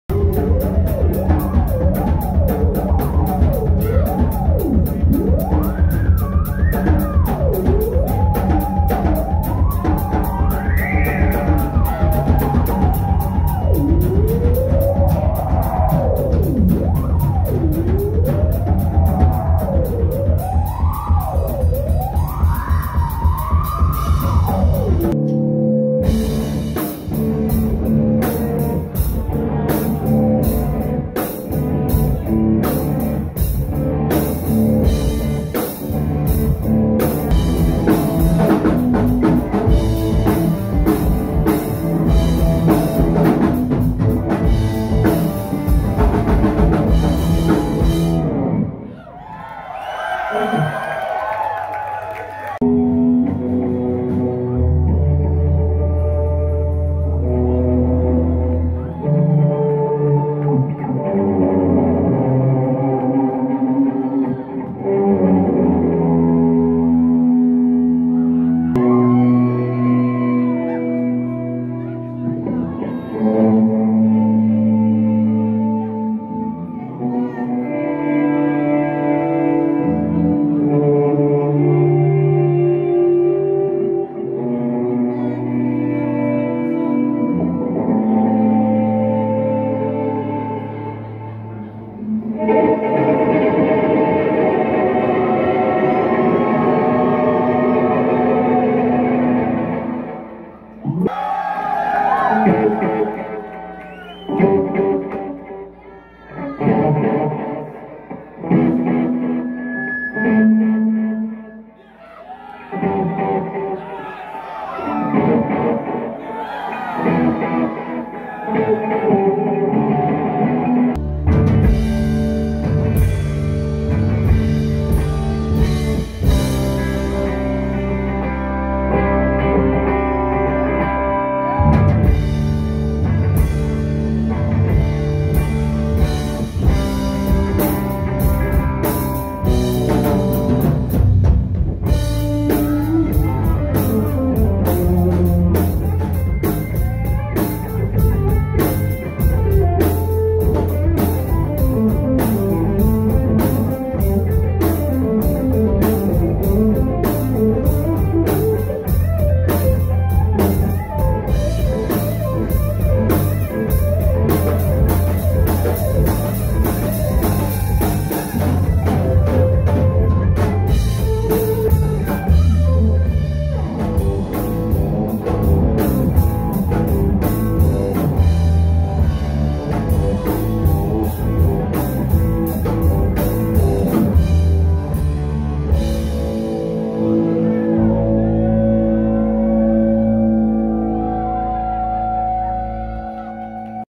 Chris Manning at Byrdfest 21 with Led Zeppelin Tribute
Highlights from performing with Led Zeppelin tribute in Ozark, Arkansas on September 19th.